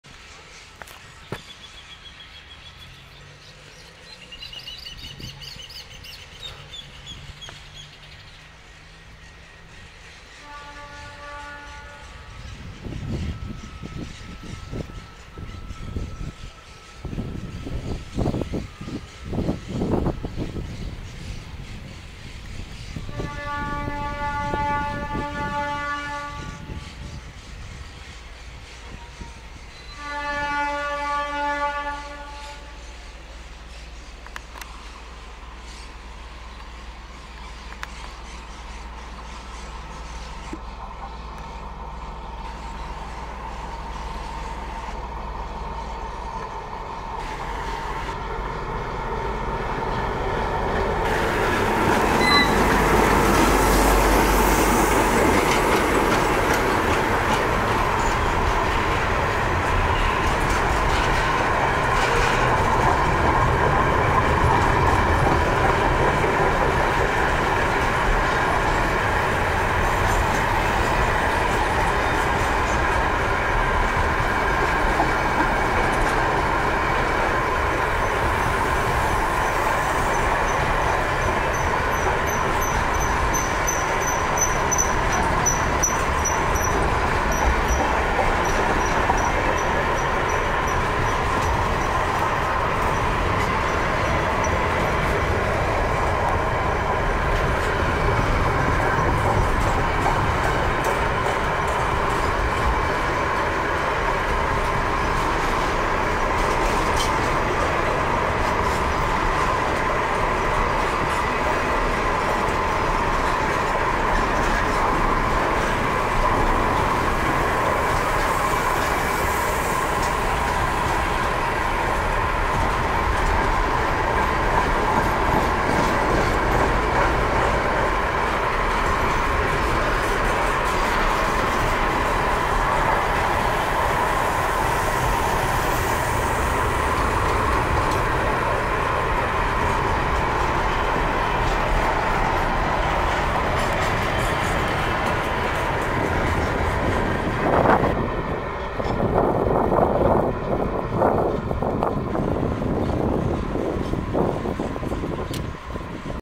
Portacontenedores vacio con GR12 6629/ALCo 8242 por San Nicolás (0496) 21-1-2023
Portacontenedores vacio con GR12 6629/ALCo 8242 por San Nicolás
Empty container ship with GR12 6629/ALCo 8242 through San Nicolás